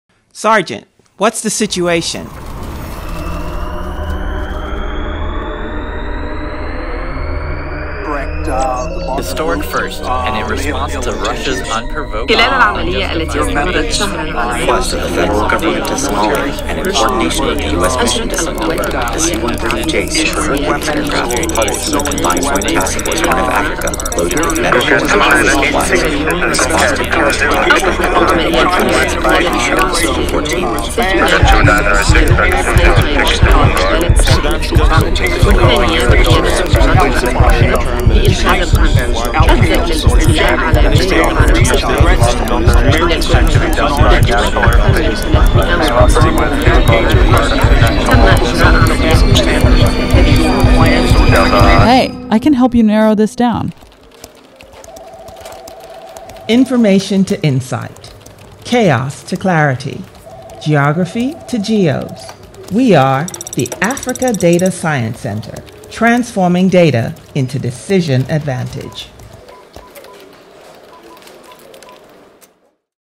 ADSC: Building Army Intelligence Capabilities in Constrained and Contested Environments
The ADSC, a pilot program begun in 2018, uses machine learning, advanced anaytics, data visualiztion, and intelligent domain expertise to build products to help analysts sort through data quickly.

Join us live at AUSA2022 for the U.S. Army WarriorsCorner: Building Army Intelligence Capabilities in Constrained and Contested Environments or watch live